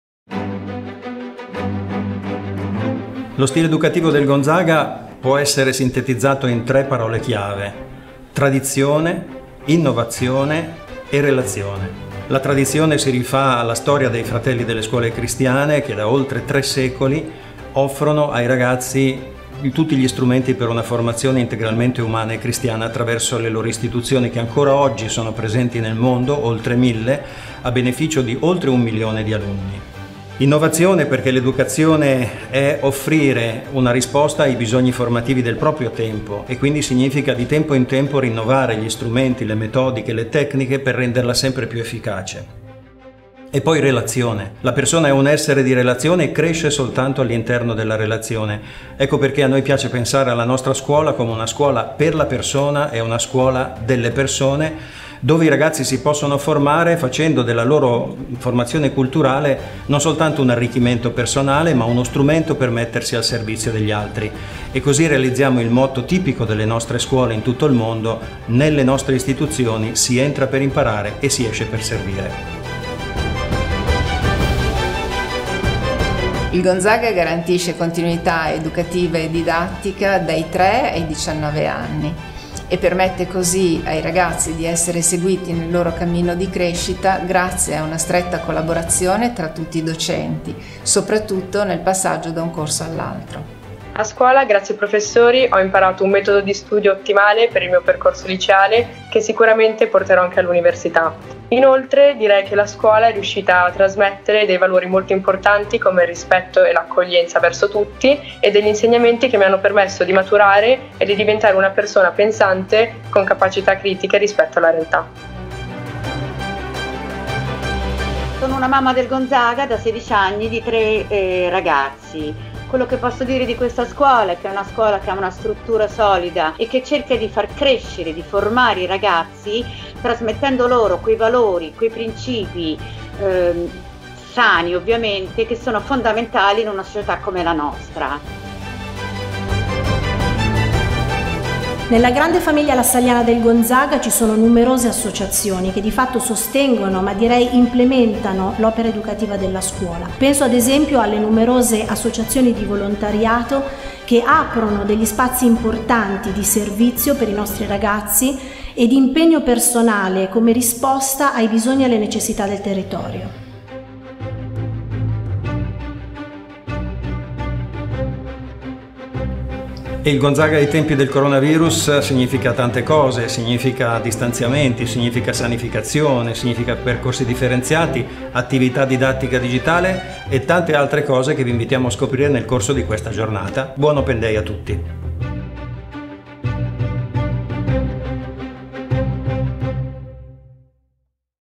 Istituto Gonzaga - Presentazione Digital Open Day 2020
L'Istituto Gonzaga è una scuola cattolica paritaria fra le più importanti del capoluogo meneghino, attenta da oltre 100 anni alla qualità dello studio e alla formazione dell'alunno secondo la plurisecolare tradizione dei Fratelli delle Scuole Cristiane.

L'Istituto - accreditato come Cambridge International School - comprende Scuola dell'Infanzia, Primaria, Secondaria I° grado, Liceo Classico, Liceo Scientifico, Liceo Europeo con due indirizzi di studio: linguistico moderno e giuridico-economico.